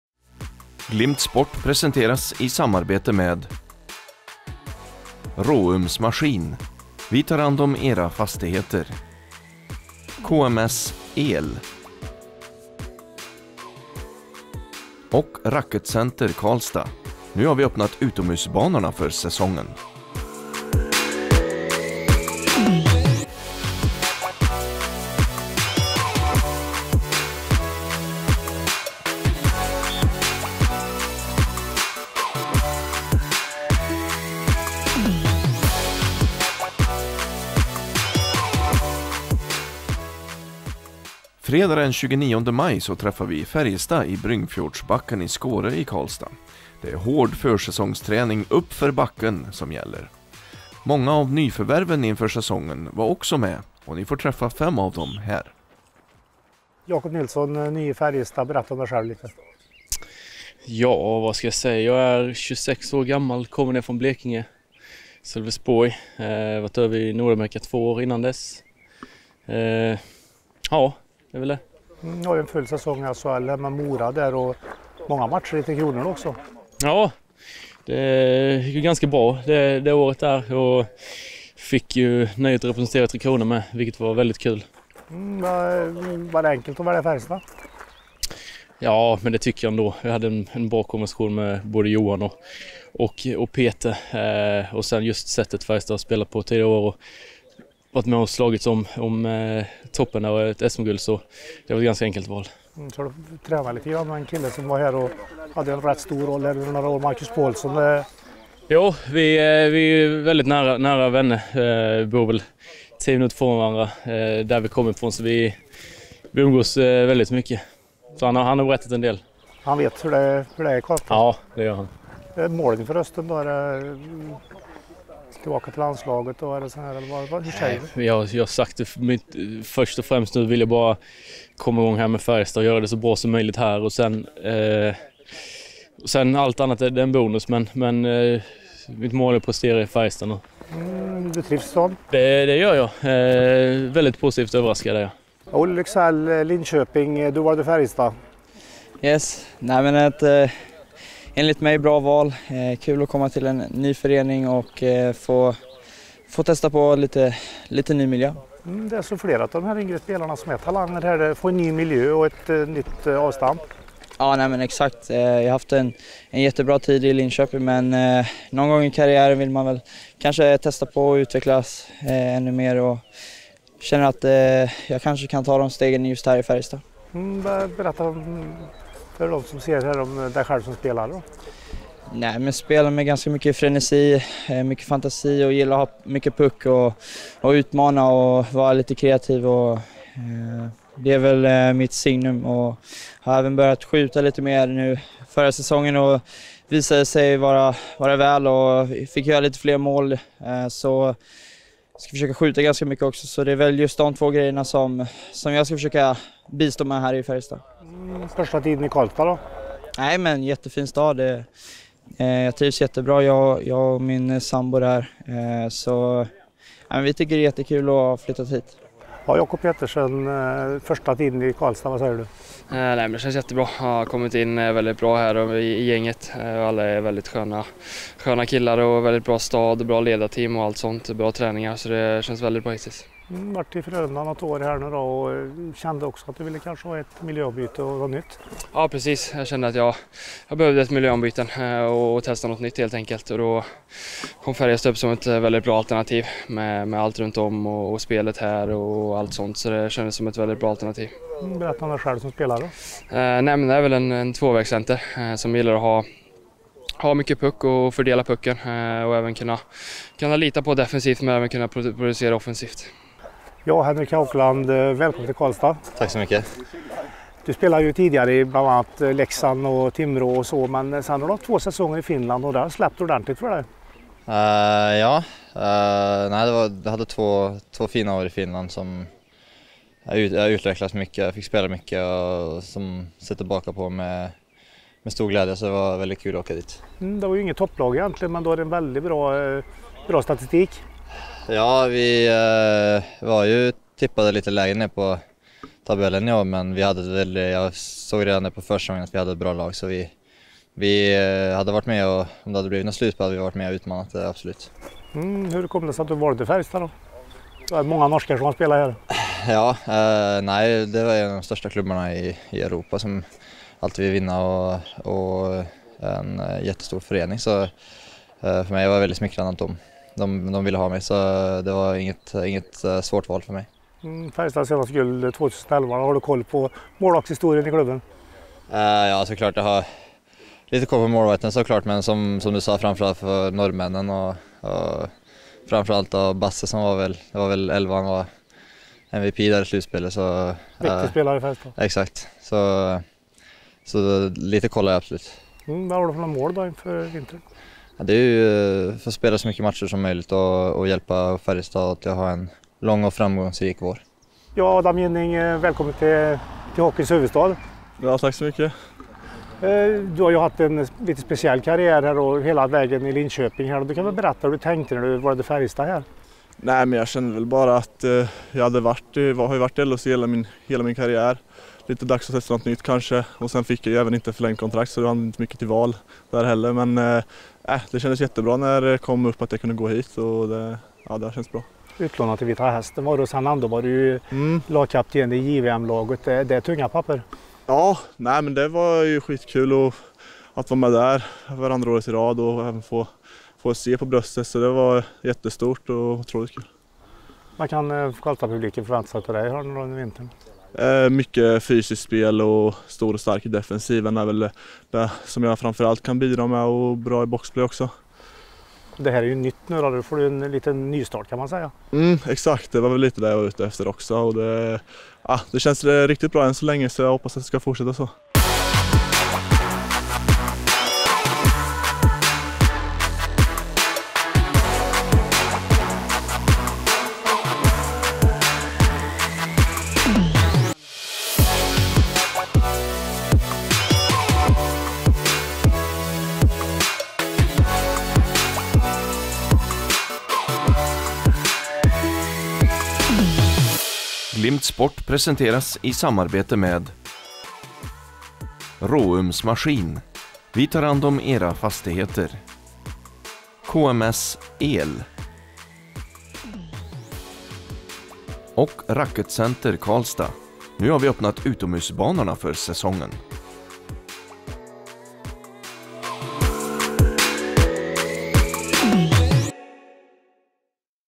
Vi träffar nyförvärven i Färjestad
Fredagen den 29 maj träffade Glimt Sport Färjestad i Bryngfjordsbacken i Skåre i Karlstad. Det var hård försäsongsträning uppför backen som stod på schemat. Många av nyförvärven inför säsongen var med och ni får träffa fem av dem här, bl.a. den nye norske målvakten Henrik Haukeland.